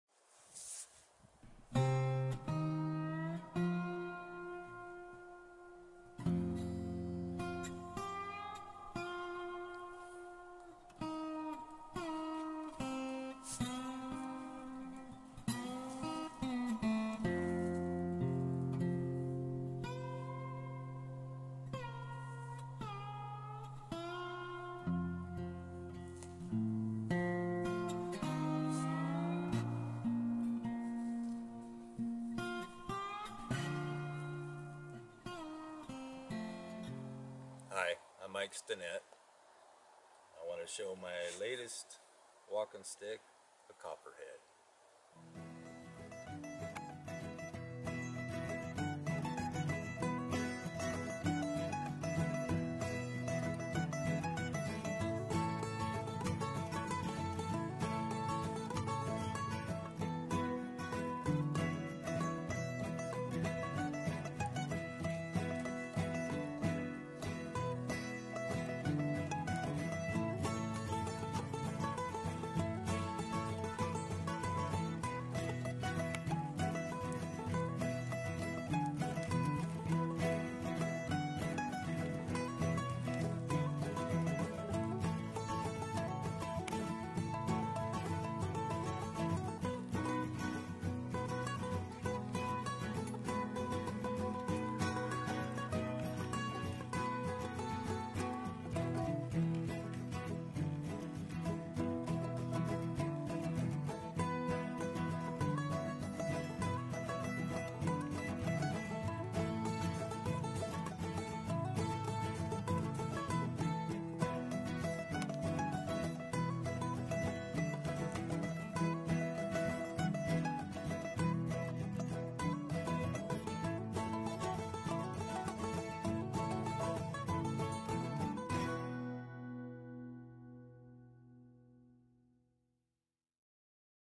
Copperhead Snake Walking Stick #38
Today I took my latest walking stick for a test hike. I carved this snake wrapped walking stick from a single piece of Lodgepole Pine.

* TOOLS I USE * (paid link)

CUSTOM ORDERS

The Making of Wood Carved Rattlesnake Walking Cane #43